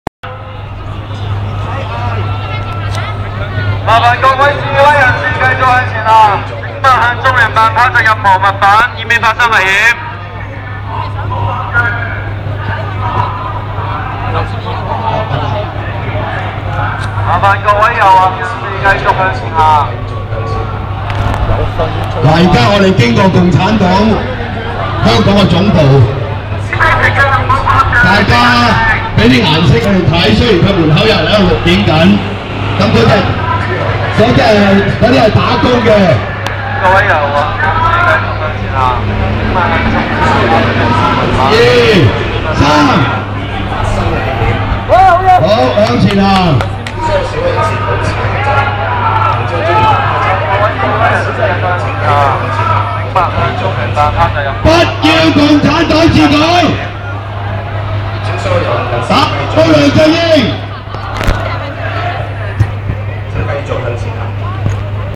人民力量反梁振英當選遊行(一支持者在門外放竹蜻蜓) 2012年3月25日
人民力量反梁振英當選遊行，支持者到達中聯辦門外，向中聯辦放外放竹蜻蜓，喻意飛起小圈子選舉。